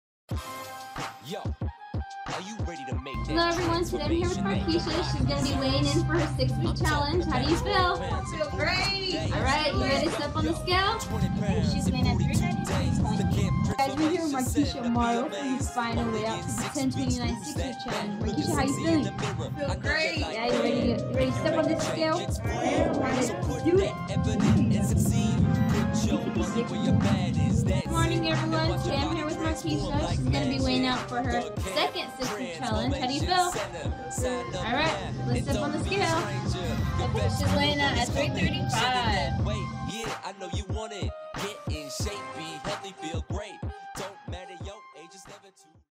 Oxnard Weight Loss Fitness 12 Week Challenge Results - Markeshea Morrow
We will now check the result of Markeshea's 12 week weight loss challenge!